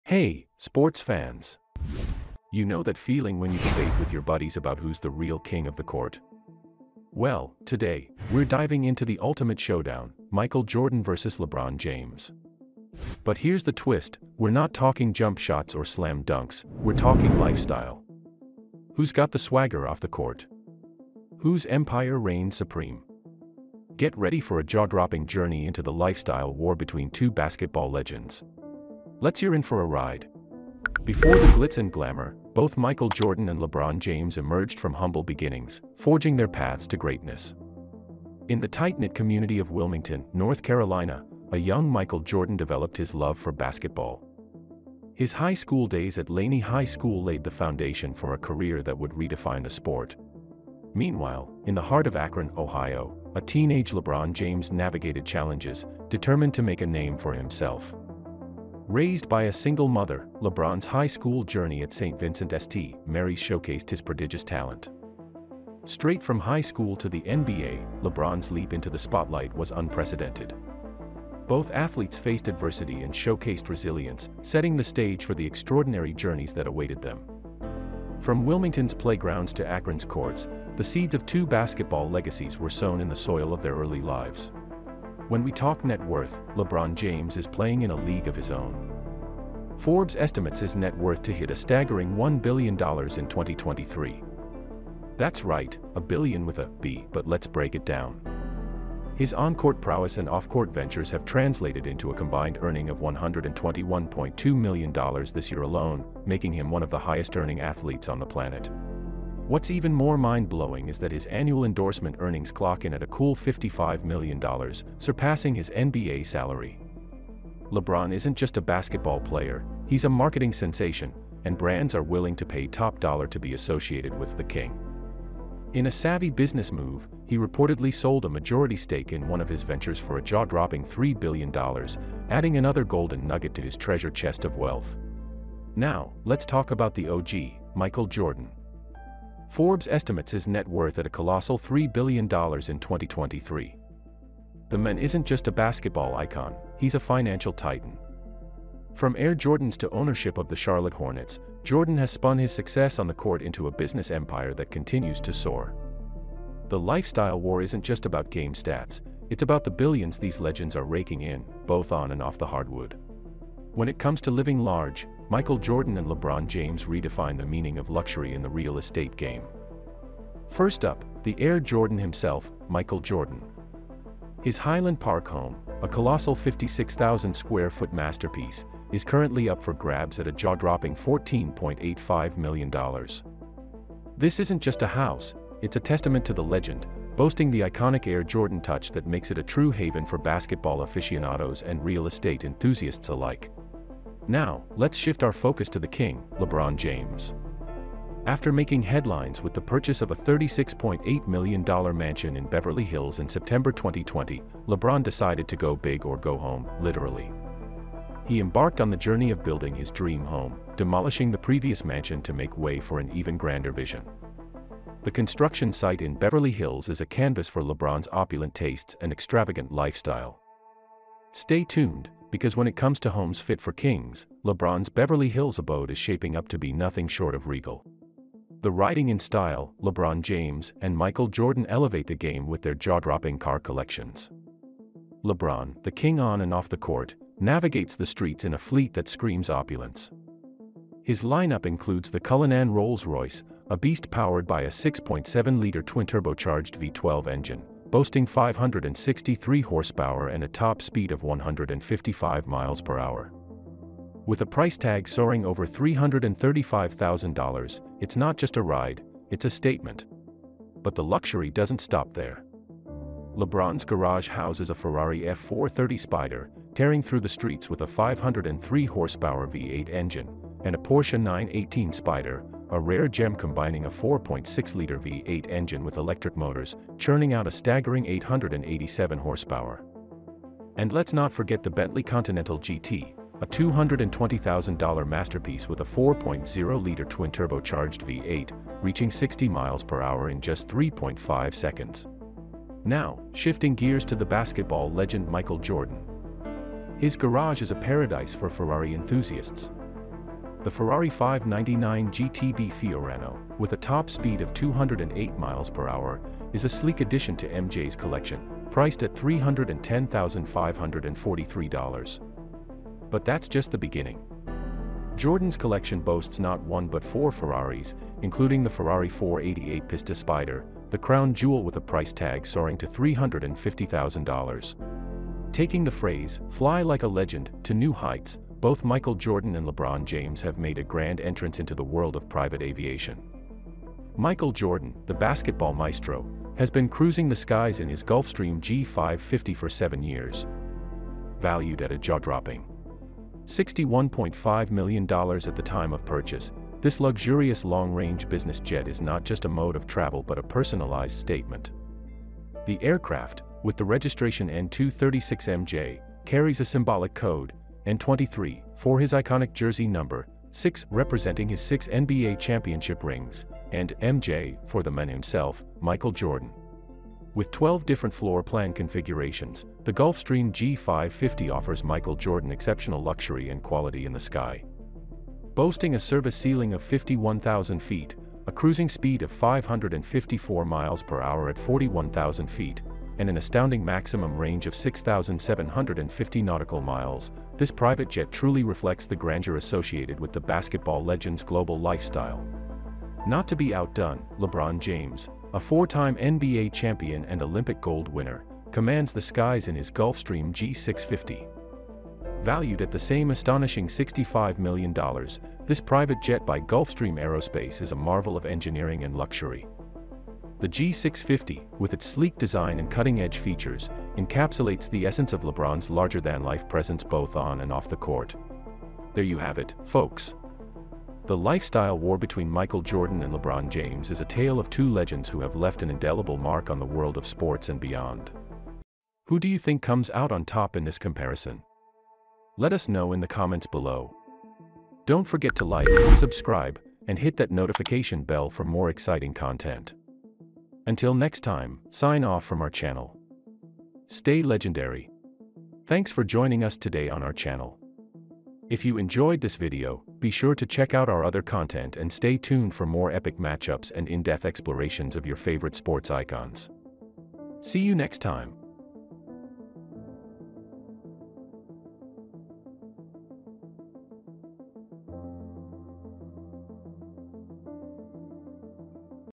Michael Jordan VS LeBron James - Lifestyle War
🔥 Dive into the extravagant lifestyles of two basketball legends, Michael Jordan and LeBron James! 🌍✨ Who reigns supreme off the court? Join us in this exclusive showdown as we explore their mansions, cars, fashion, and more.

In this video, we're comparing and contrasting the lifestyles of Michael Jordan and LeBron James.

What does this lifestyle war between Michael Jordan and LeBron James mean for the sport of basketball? You'll have to watch the video to find out! This video is full of interesting insights and comparisons that will make you a better sports fan.

📈 Why Watch?
Discover the untold stories behind the lifestyles of Michael Jordan and LeBron James and join the debate on who truly holds the crown in the lifestyle arena. Don't miss out on the insights and surprises that make this comparison a must-watch for basketball enthusiasts and lifestyle aficionados alike!